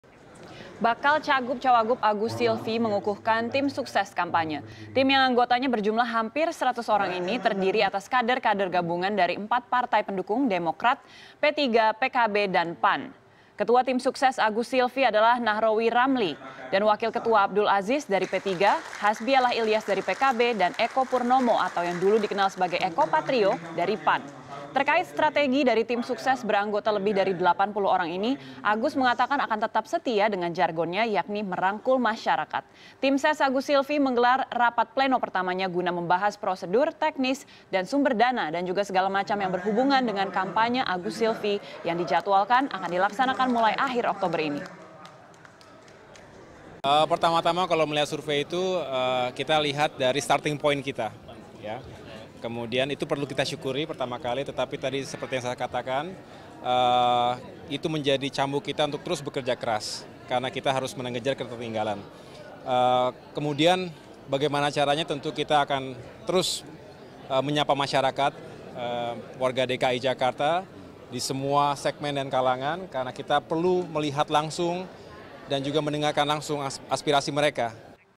Pengukuhan Tim Sukses Agus - Sylvi
Cagub - Cawagub Agus-Sylvi mengukuhkan tim sukses kampanye. Tim yang anggotanya berjumlah hampir seratus orang ini terdiri atas kader-kader gabungan dari empat partai pendukung, Demokrat, PPP, PKB, dan PAN. Pengukuhan tim sukses pasangan calon Gubernur dan Wakil Gubernur DKI Jakarta, Agus Harimurthi Yudhoyono dan Sylviana Murni, dihadiri segenap anggota tim, yang merupakan gabungan dari empat partai pendukung, antara lain partai Demokrat, PPP, PKB, dan PAN.